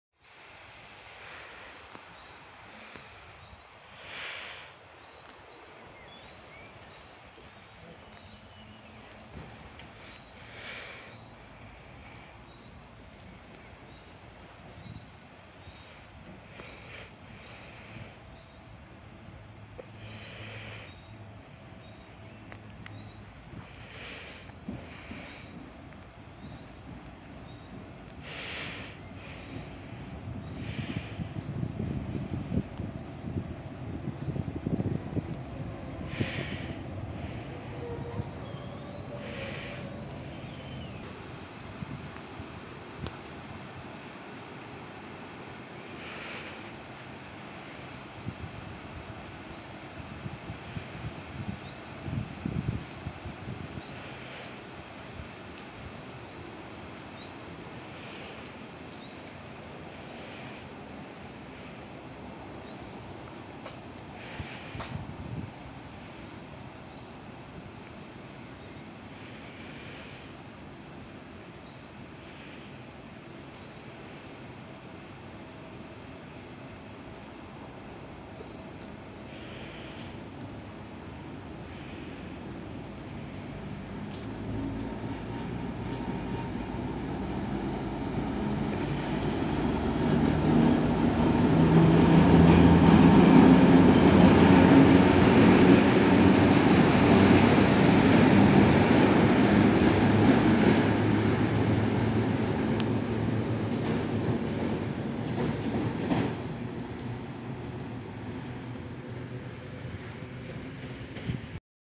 Luzak ST44-1221 w stronę Zielonej Góry , 311D-03 na bocznicy | Żary
O POCIĄGU:
Nazwa: ST44 311D
Trasa: Goerlitz  → Zielona Góra Główna
Wagony:brak